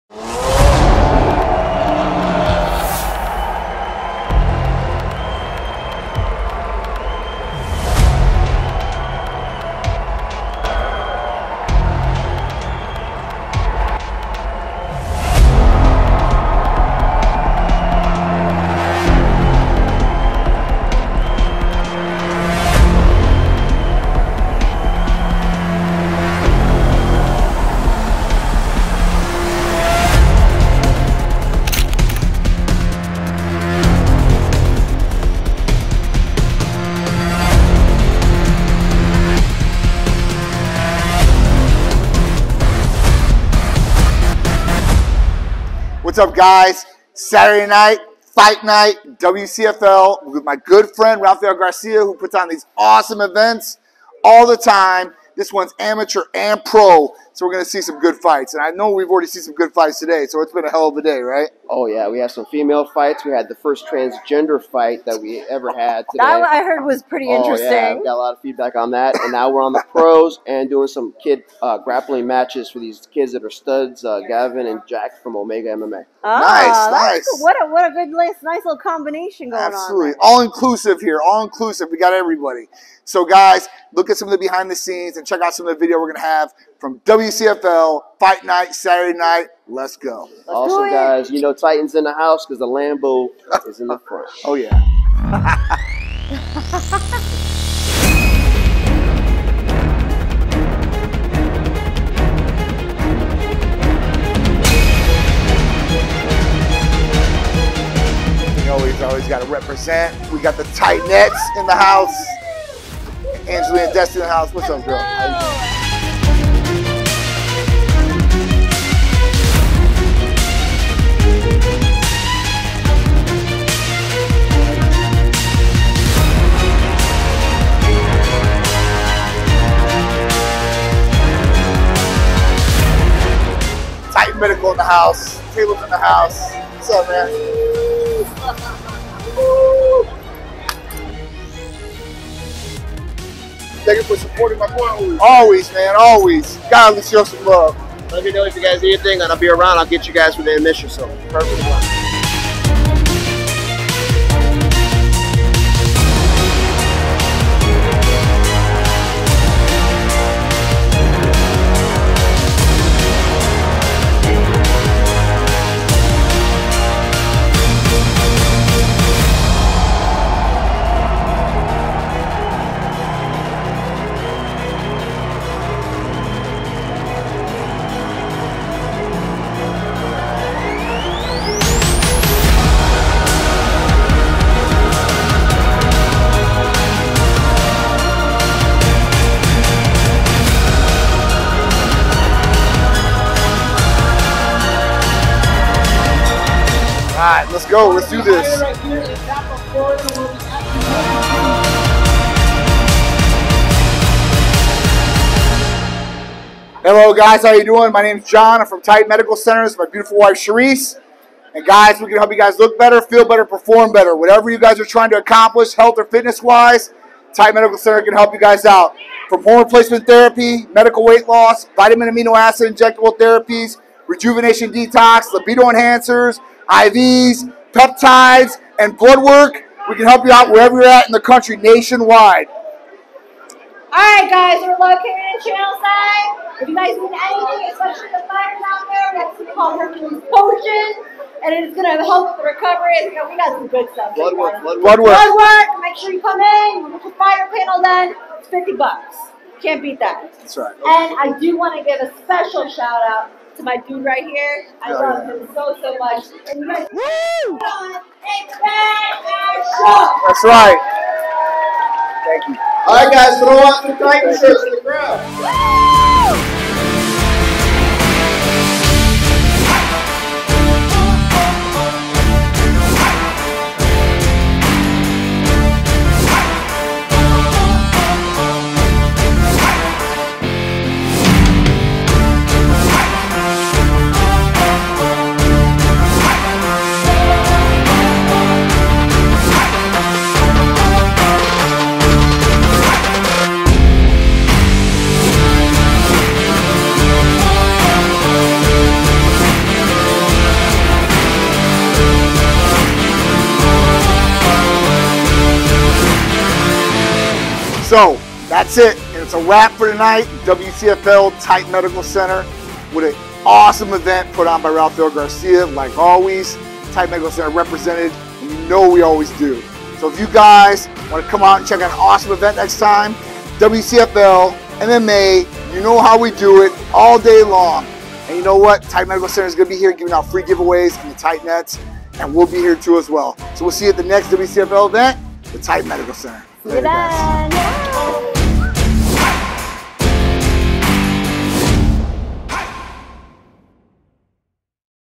Titan at the WCFL 31 (World Class Fight League) amateur & professional MMA fights here in Tampa!
Great job to Rafael Garcia and his crew to another great combat sports event here in our hometown. We had John, Sharisse, Big Dru and even Jason Skeldon at this fun event. See you at the next one!